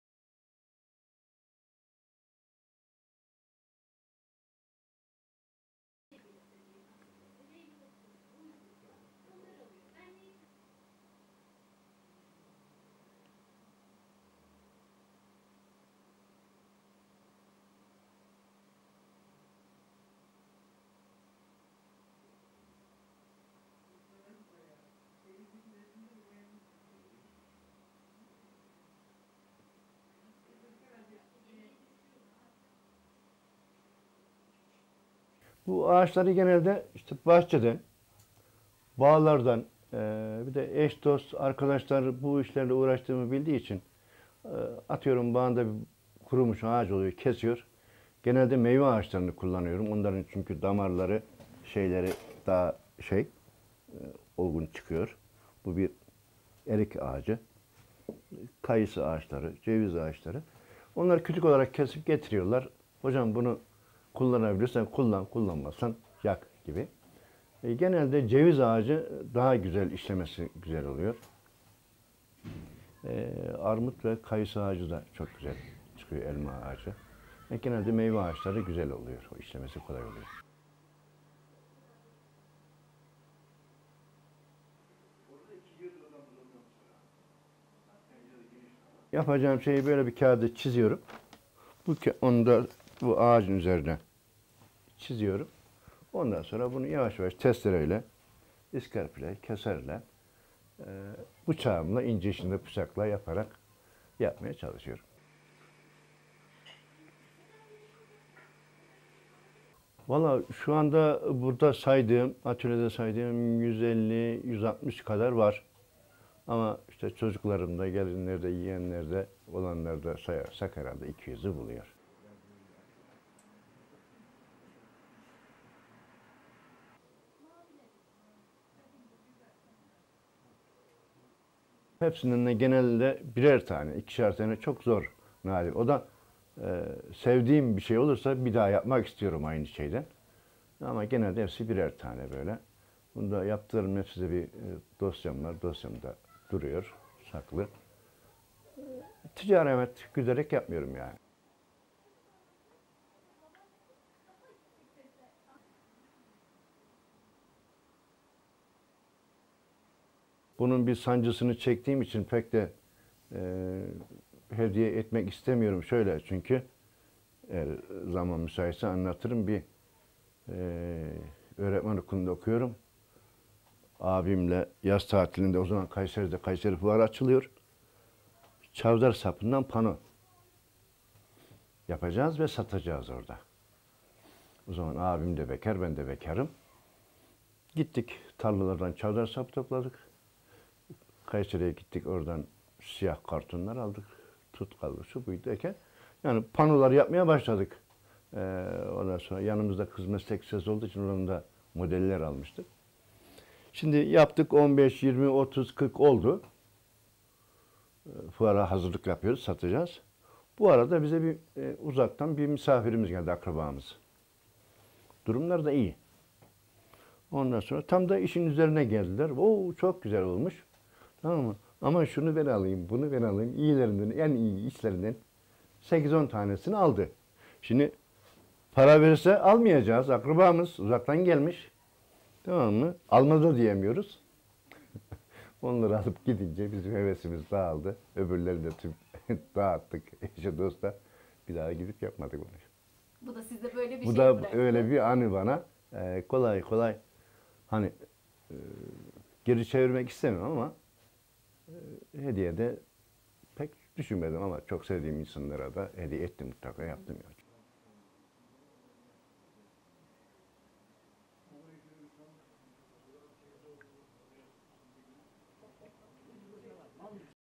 Kurumuş ağaçlar emekli öğretmenin elinde sanata dönüşüyor
Kayseri'de yaşayan 70 yaşındaki Yılmaz Güler, öğrenciyken gönül verdiği ağaçtan heykel yapma hobisine emekliğinde de devam ediyor Yılmaz Güler: "Üzerimde sürekli çakı, bıçak vardır, oturduğum yerde ağaçla uğraşırım" "Tüm arkadaşlar, eş dost, bir sergi açmamı söylüyor. Benim böyle bir deneyimim yok, nasıl olur bilmiyorum. Geçen yıl ciddi ciddi düşünmeye başlamıştım, pandemi araya girdi. Bir sergi açayım isterim, herkes görsün, illa meraklıları vardır"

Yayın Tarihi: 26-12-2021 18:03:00